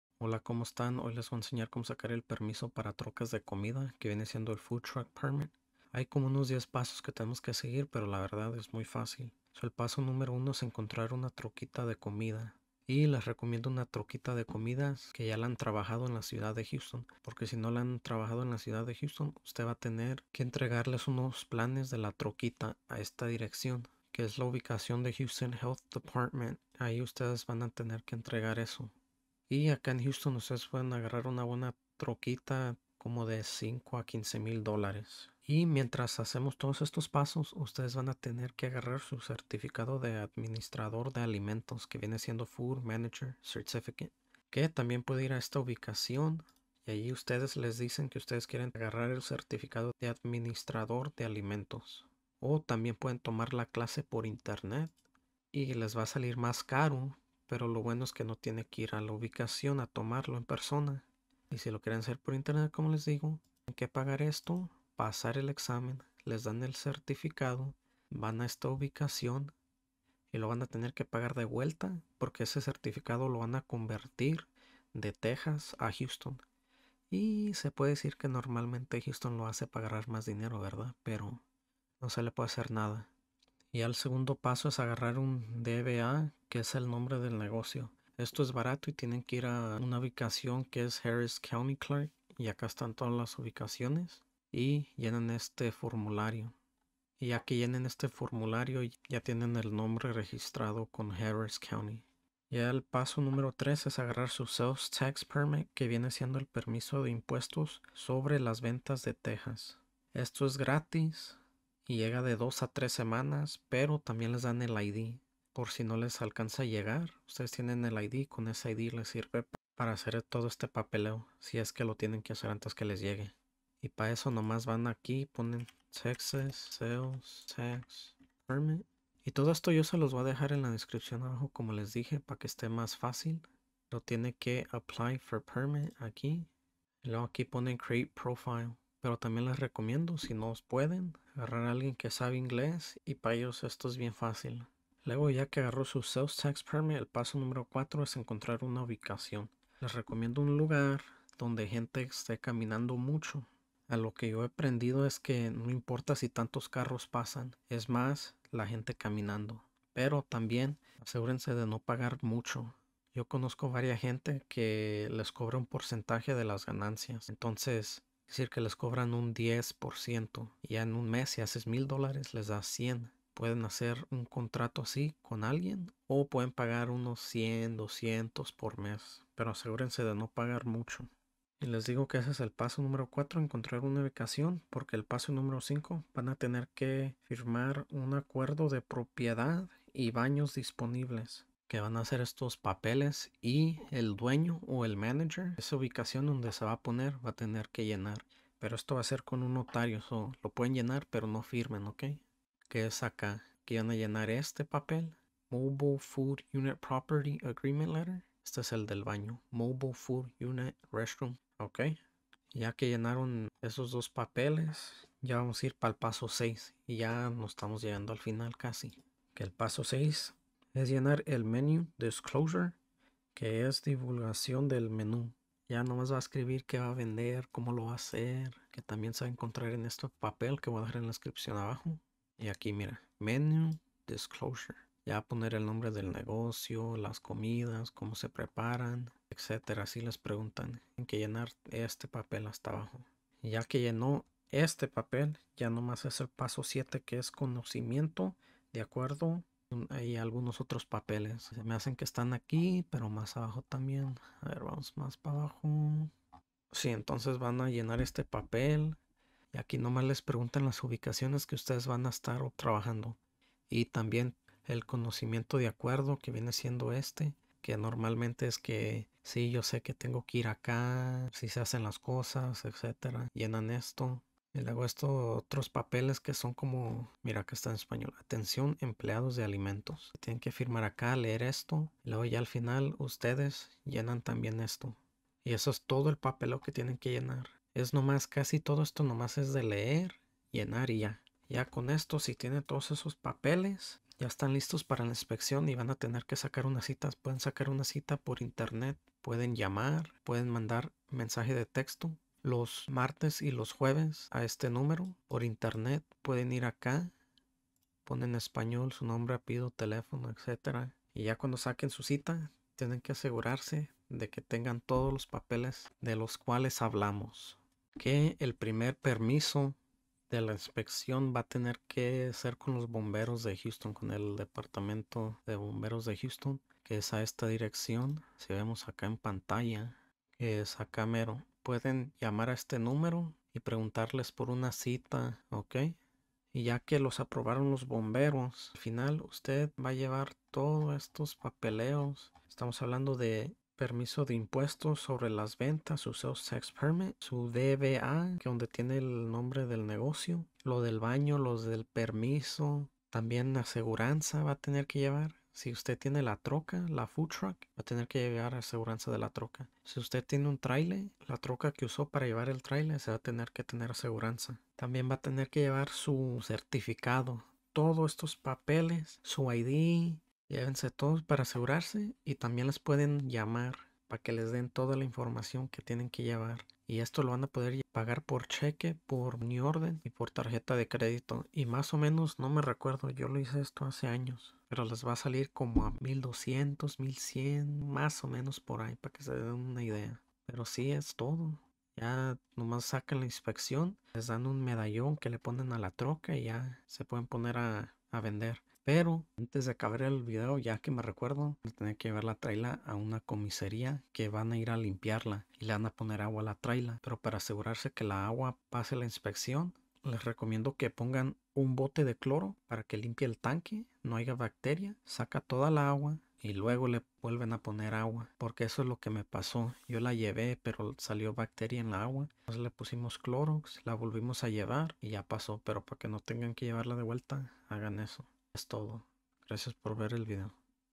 COMO AGARRAR PERMISOS Y LICENCIAS PARA CAMIONES DE COMIDA / ALIMENTOS / REMOLQUES EN HOUSTON HARRIS
En este video les muestro como consiguir / agarrar todo los permisos y licencias para su camion de comida aqui en Houston el condado de Harris.

As clic en el enlace de arriba y luego coundo se abras la pagina vuelves a ser clic donde dice "Required documents checklist with forms" para agarrar todo los formularios.

Guia para estudiar para el certificado de administrador de alimento (food manager) - Esta en Ingles:

Formulario DBA (lo tiene que llevar a una de las ubicaciones de Harris County Clerk's Office):

No estoy asociado de ninguna manera o forma con Houston Health Department or Houston Fire Department. Esta es solo información tomada de su sitio web y entregada al espectador bajo su propio riesgo. Esto no es un consejo legal / financiero.

Obtenga acciones gratis en la bolsa de valores cuando se registre con los enlaces a continuación:

Gracias!